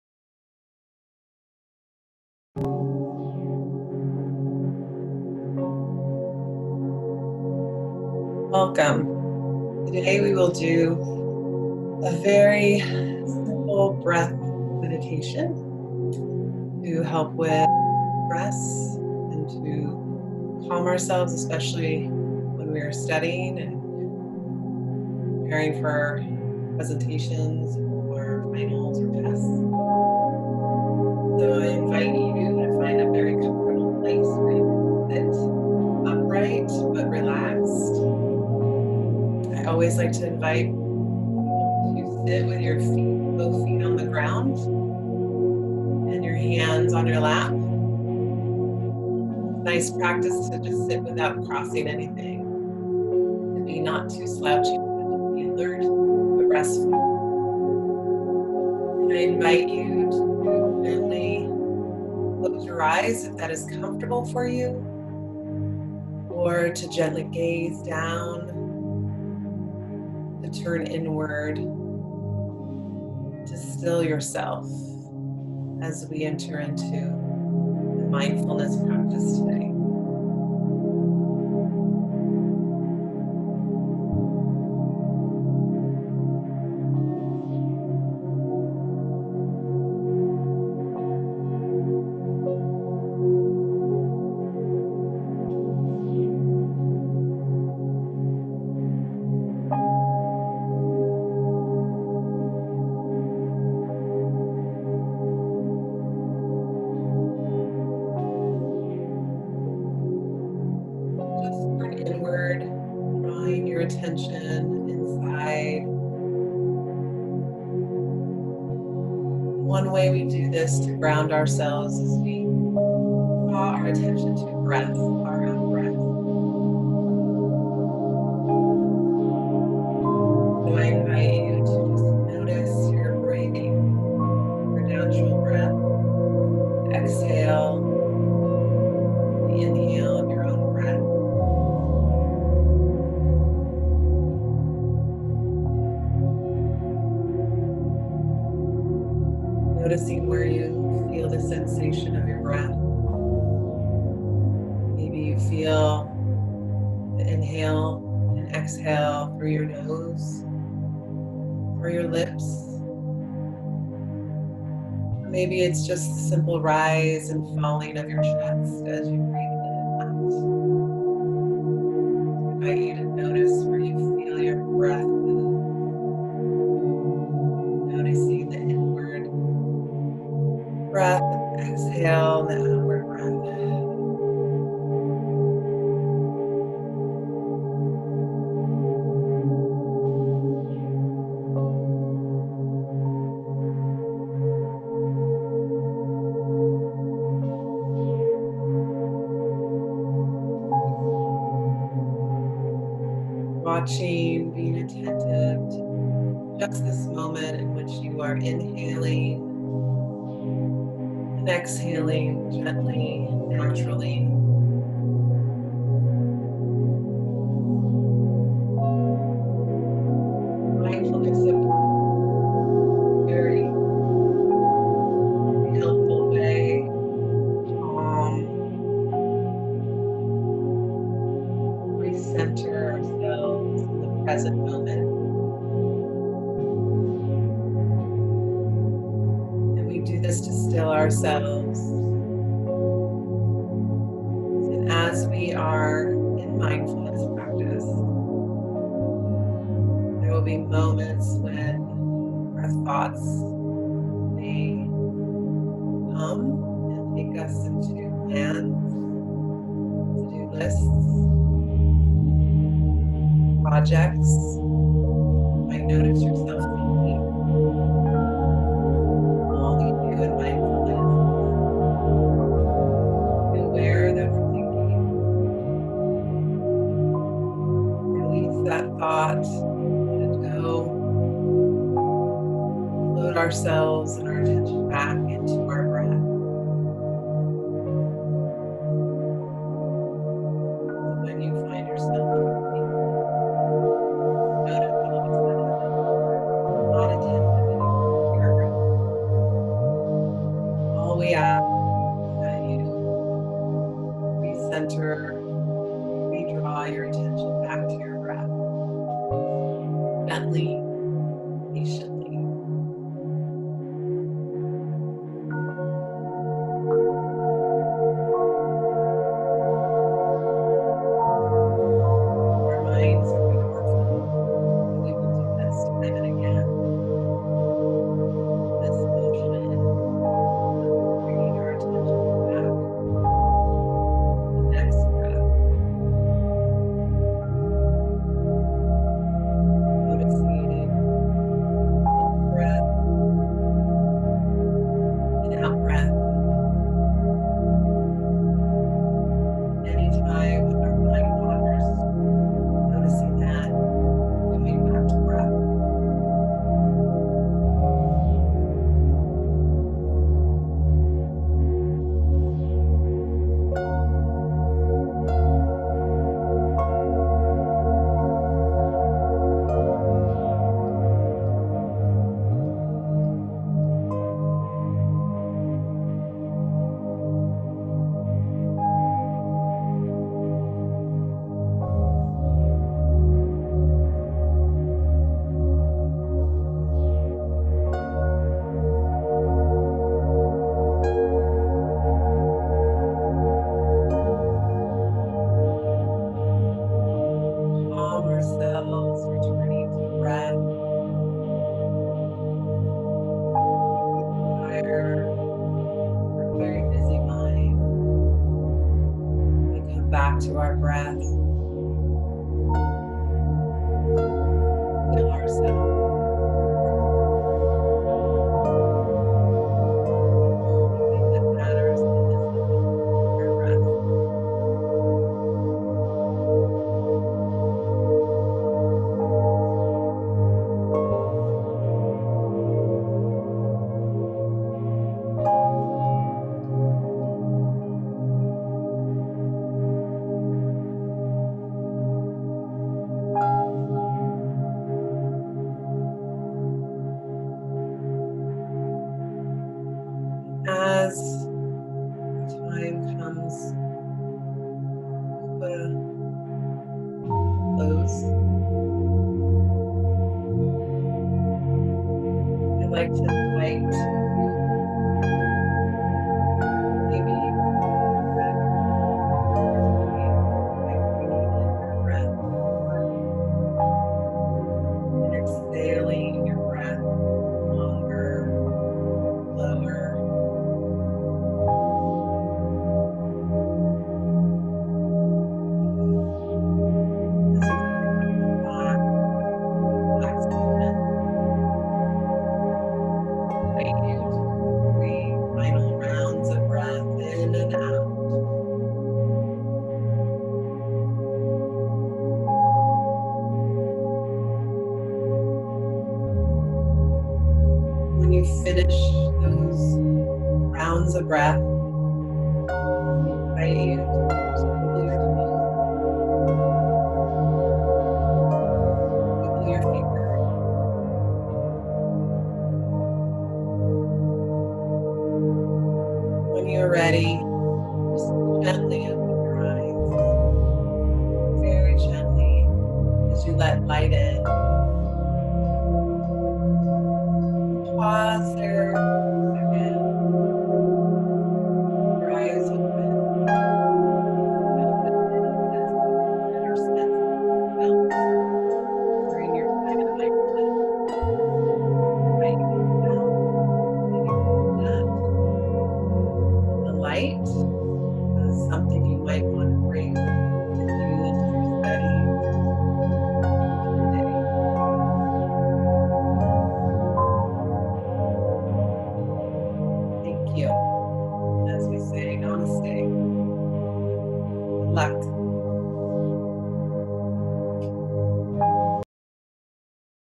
10-minute Mindful Breath Meditation
Feeling down or anxious? Try this 10-minute Mindful Breath Meditation from HNU Co-Director for Campus Ministry Jenny Girard Malley.